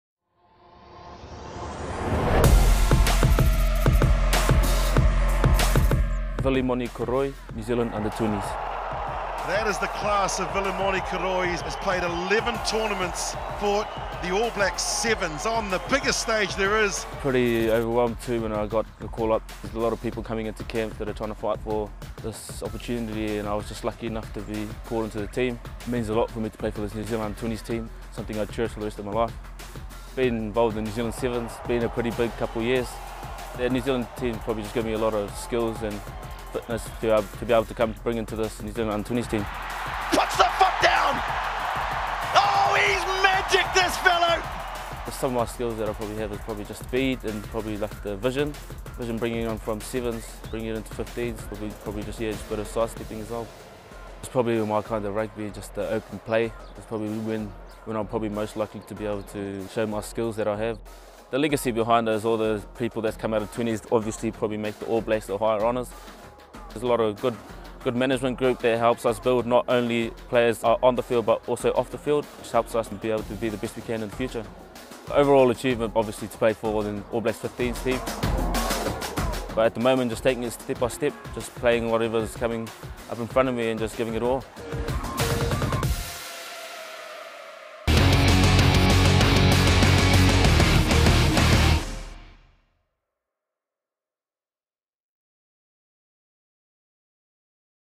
Vilimoni Koroi | The hot-stepping star
He's been doing it on the sevens series all year but now the New Zealand flyer, Vilimoni Koroi, is looking to announce himself on the world stage at the World RugbyU20 Championship